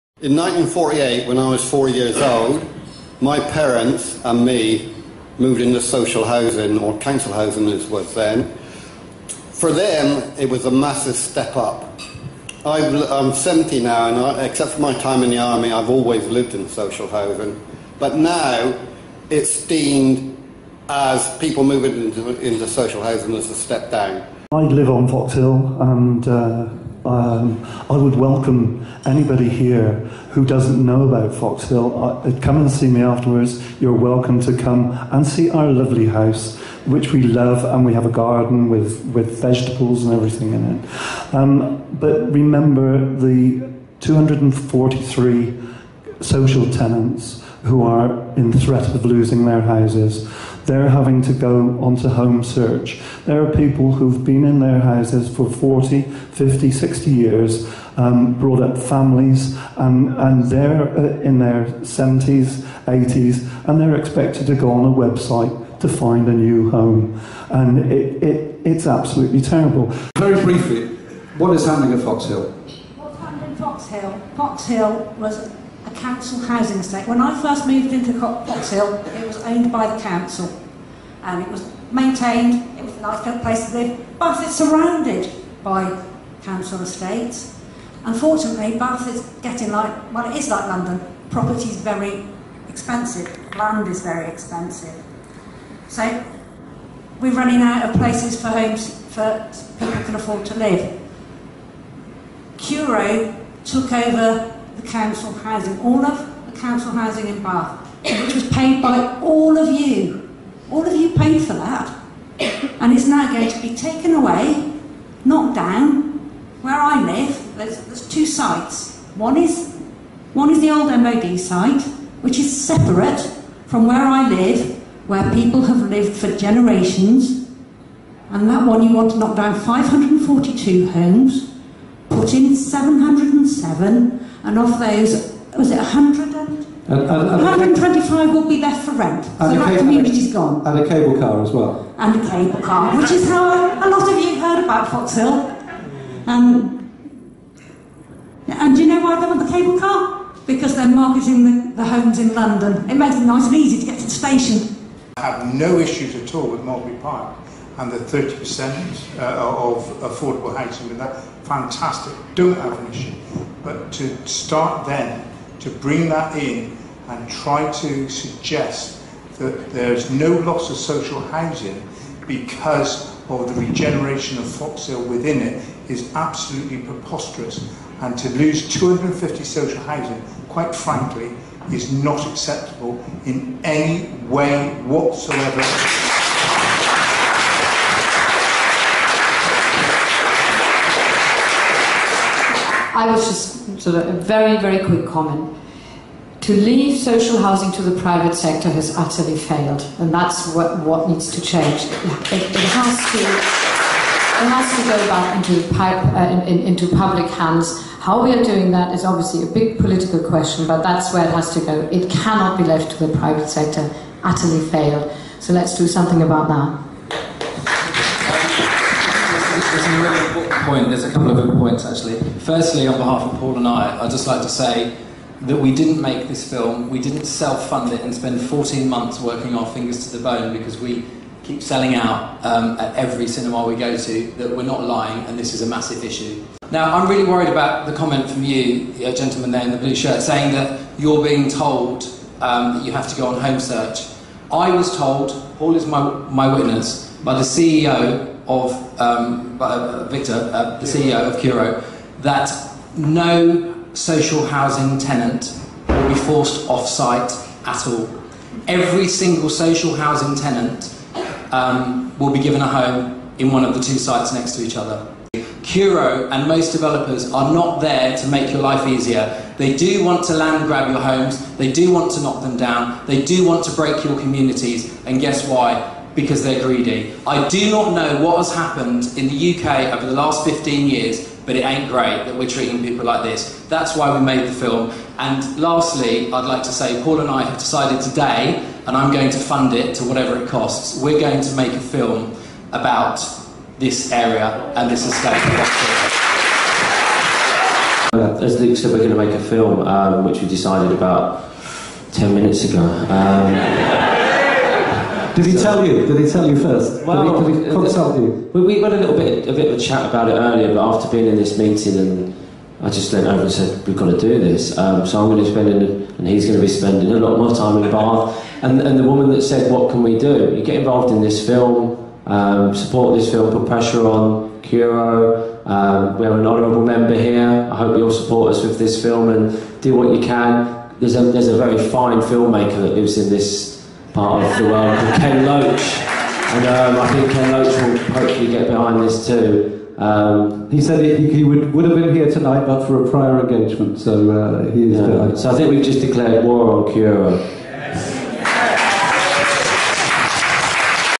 Q&A highlights following screening of "Dispossession - The Great Social Housing Swindle"
Foxhill residents speak out at the Q&A session following the screening of "Disposession - The Great Social Housing Swindle" in the Widcombe Social Club on Friday 7th July 2017, sponsored by the Widcombe Association and Bath Film Festival.

Speakers: a resident of Foxhill; David Beaven, another resident; Nola Edwards, leader of the Save Foxhill campaign; Bob Goodman, BANES Cabinet Member for Development; Wera Hobhouse, Member of Parliament for Bath & North East Somerset; Luke Doonan, Executive Producer of "Dispossession"; Paul Sng, Director of "Dispossession".